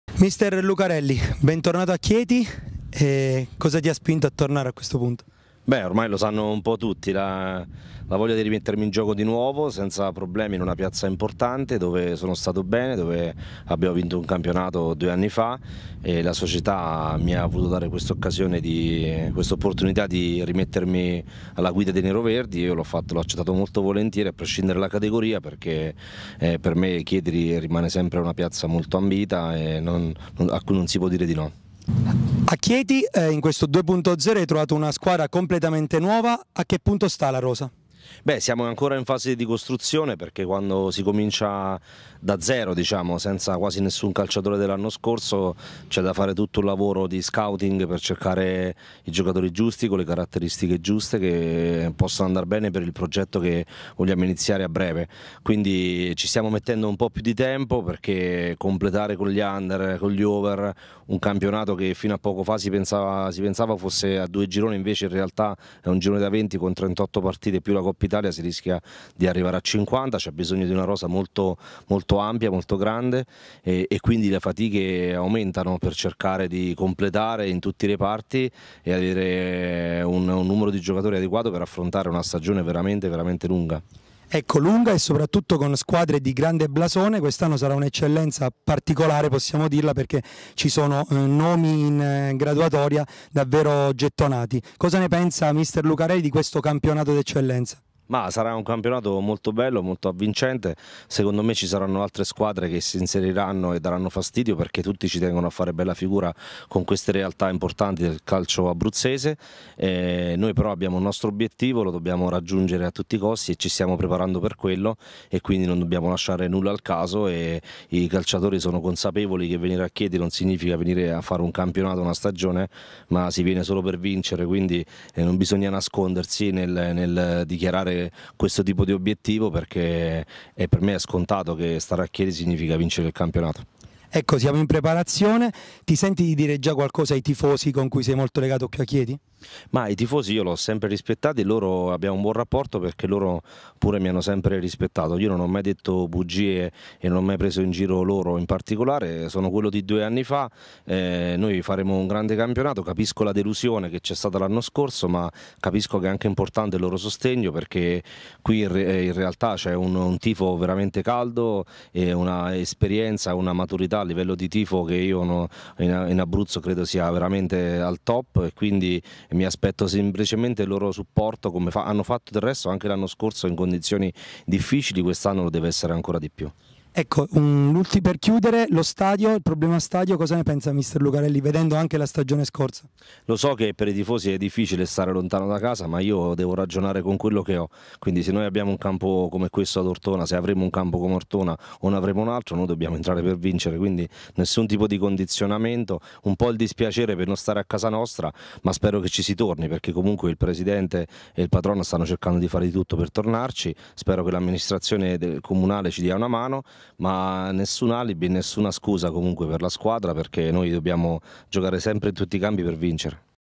Lucarelli:'Al Chieti non potevo dire di no.Qui per vincere'
"Al Chieti, al di là della categoria, non si poteva dire di no. La squadra è ancora in fase di costruzione, stiamo lavorando per affrontare al meglio la stagione". Parla così, ai canali della società, il trainer neroverde, Alessandro Lucarelli. Sul campionato:"Sarà avvincente, ma noi siamo il Chieti e vogliamo vincere".